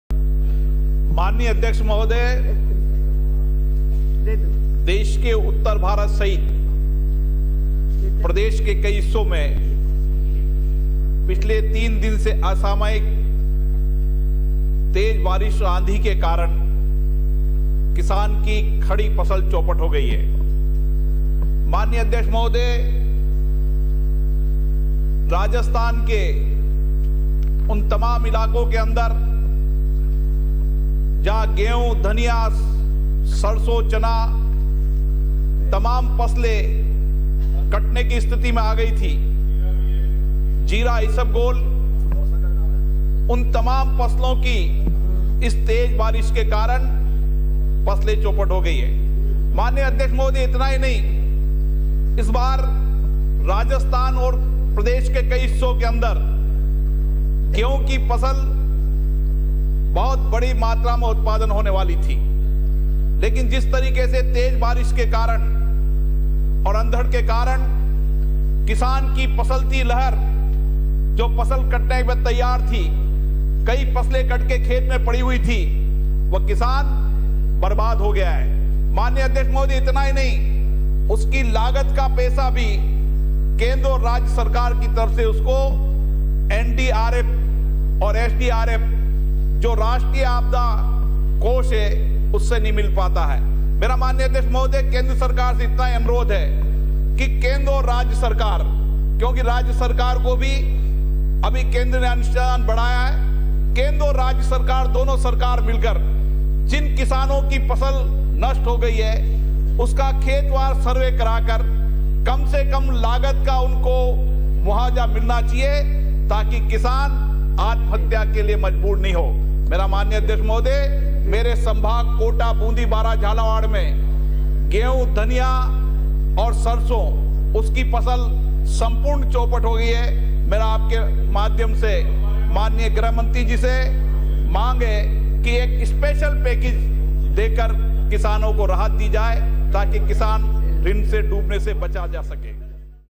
Matters of Urgent Public Importance by members during zero hour: Shri Om Birla: 02.03.2015
02 March 2015, Matters of Urgent Public Importance by members during zero hour: Shri Om Birla